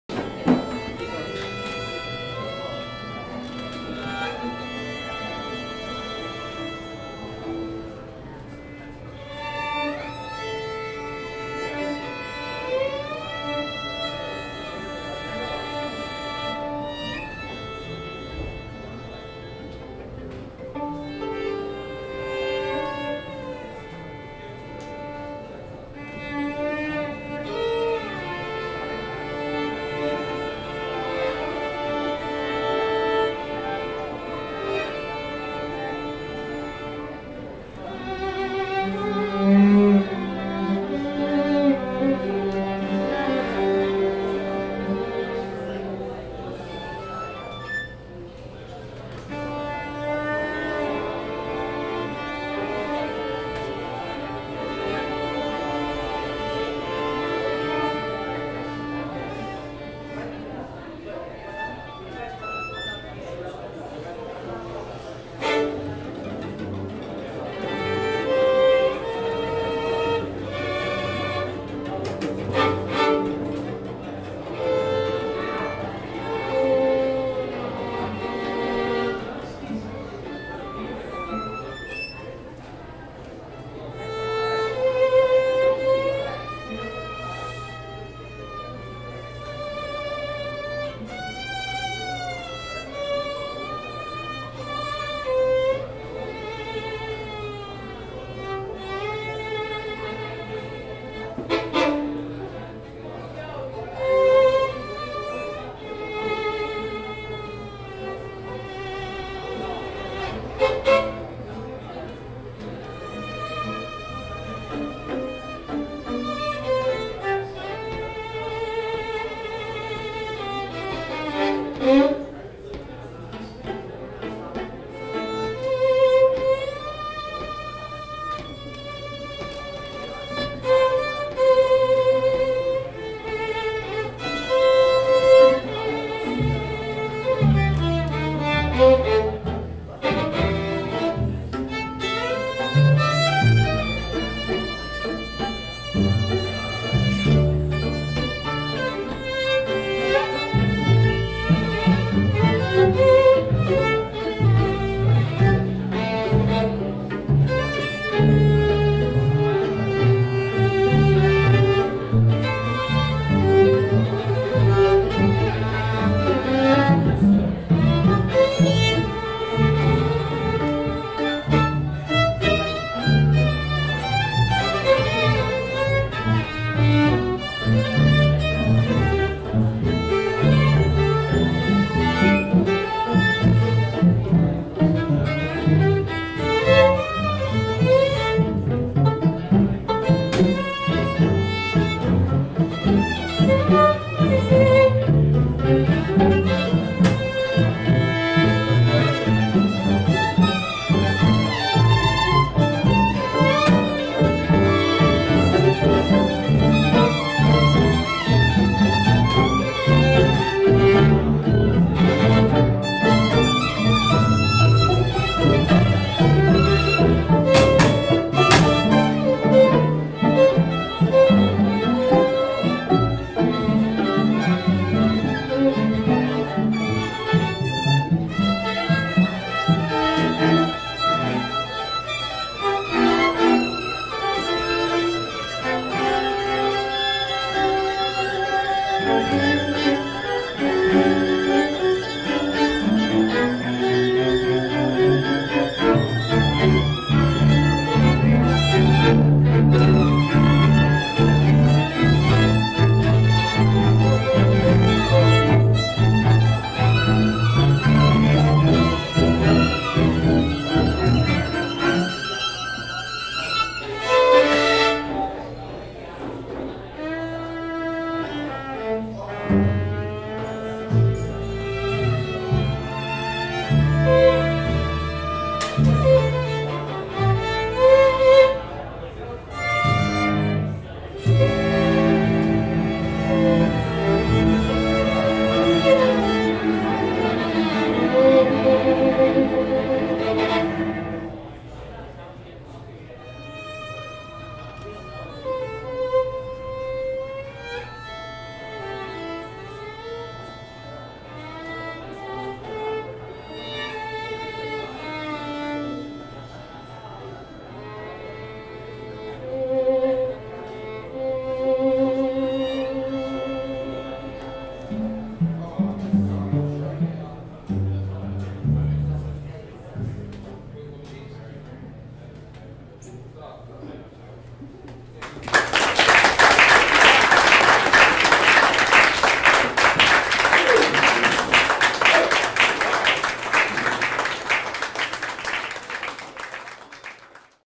SOTA @ Sweet Inspirations Cafe November 14th, 2011 pt6
Students from the SOTA orchestra perform in ensemble groups at Sweet Inspirations Cafe on Market St in SF on monday November 14th, 2011